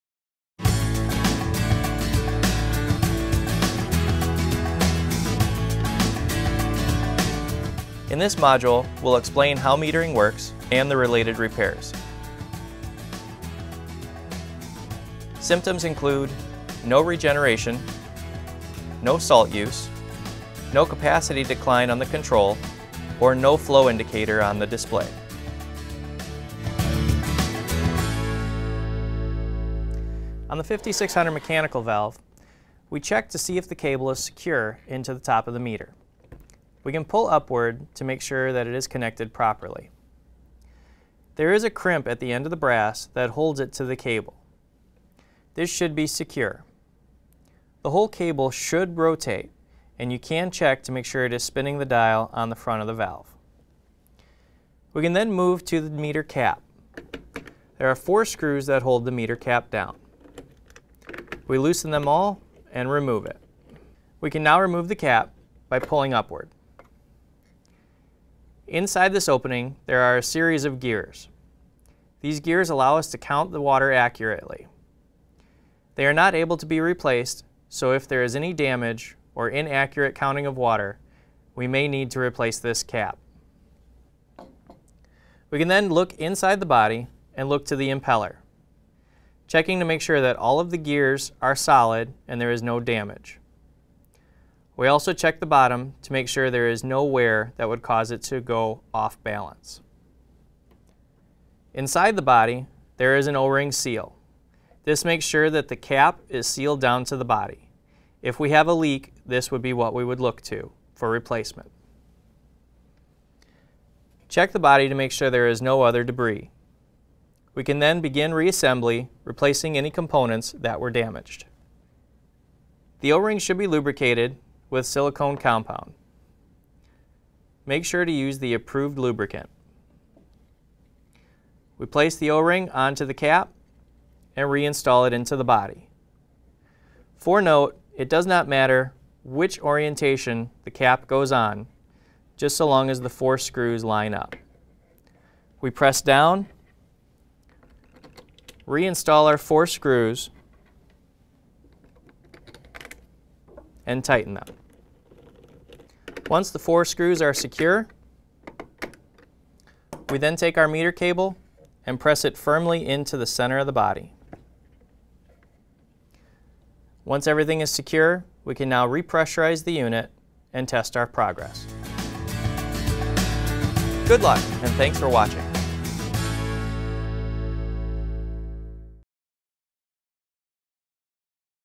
Fleck 5600 Meter Repairs
These videos are intended for the use of our loyal partners and their dealers. To find a reputable dealer in your area

This video demonstrates how metering works and the related repairs.